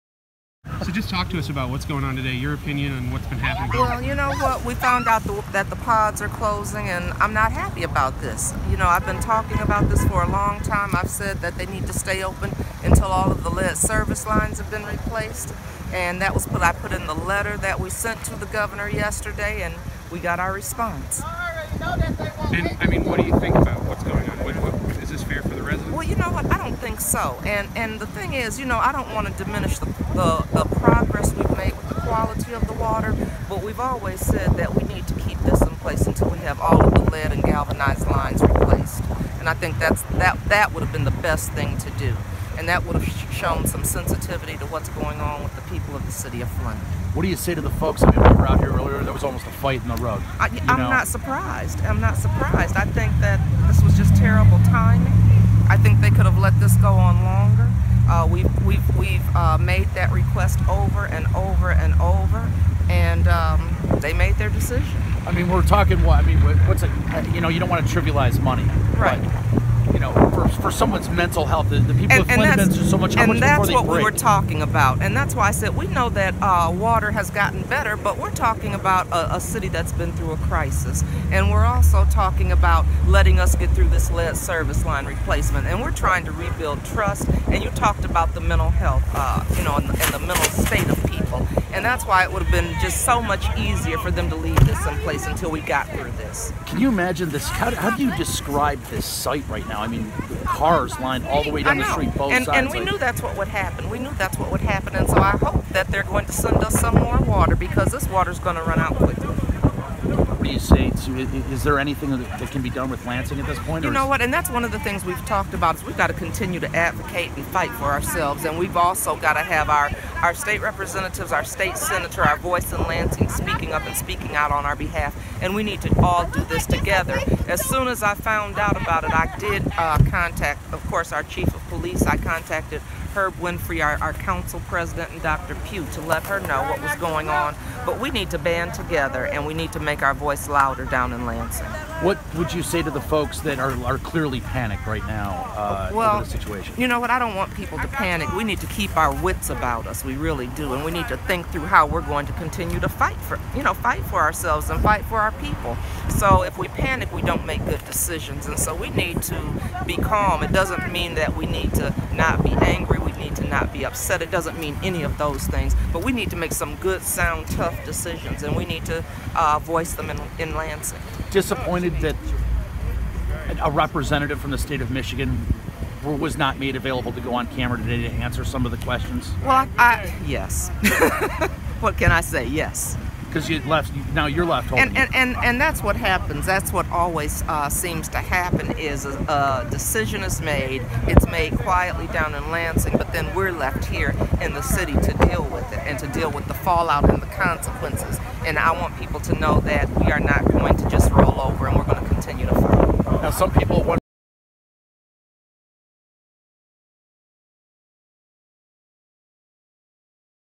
Flint Mayor Karen Weaver upset with state’s decision to close water sites
Flint Mayor Karen Weaver upset with state’s decision to close water sites, seen answering questions at one of the sites on Friday, April 6, 2018 on Dort Highway in Flint.